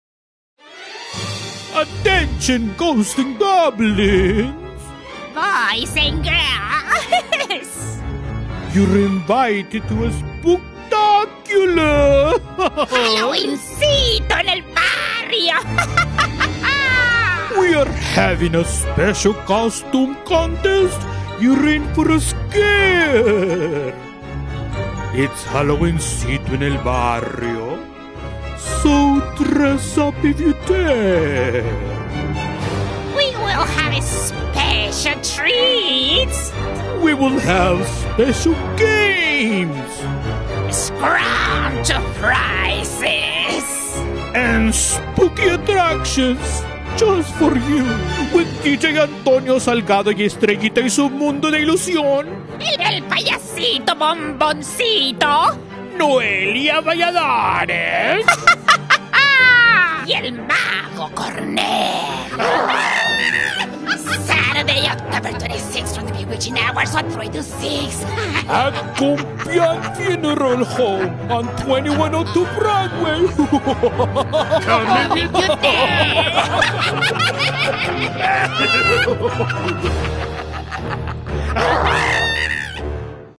Halloweencito En El Barrio Promotional Invitation in Color
HALLOWEENCITO EN EL BARRIO is coming to Houston!  We will host the ultimate Halloween experience and A.R.T.E, NHPO, TBH, ADELANTE PICTURES, ALCREJ, GOLDEN HEARTS, BUENOS DIAZ, COLOR, 2V TELEVISION, MAYAN INSURANCE, THE MENDEZ FAMILY, THE JAY DE LEON FAMILY, COMPEAN FUNERAL HOME,  KIDS RIGHT TO KNOW and HCC SOUTHEAST  invite you to bring your children to a fun filled afternoon of ghoulish entertainment by magicians, dancing clowns and musical talents.

As a collaborative mission, we strive to bring safe and fun attractions to our community while producing events such as HALLOWEENCITO EN EL BARRIO for our local residents, businesses, and non-profit organizations.

Production Company: Adelante Productions
Distribution Company: Adelante Pictures
Producer: Gregg Angel Rangel
Director: Baldemar Rodriguez